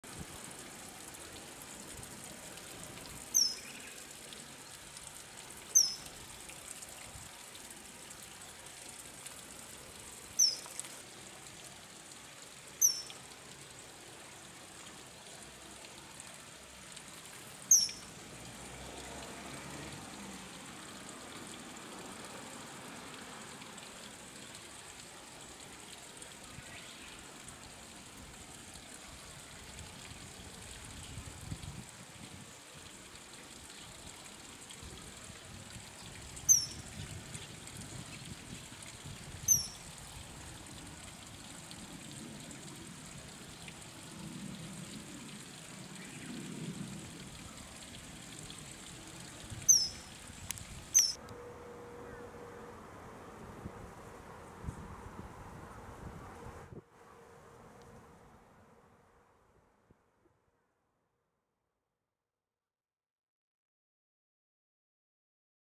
Common Kingfisher ( Alcedo atthis ) Voice and swallow a fish in May and January 2024
Kingfishers can often be seen even in urban rivers. They call with clear voices.